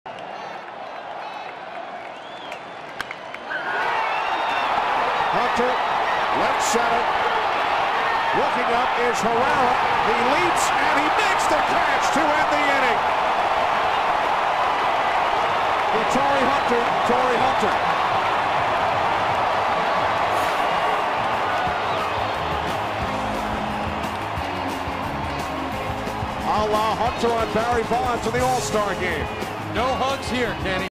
2012/06/23 Herrera's leaping catch
LAD@LAA: Herrera robs Hunter of a home run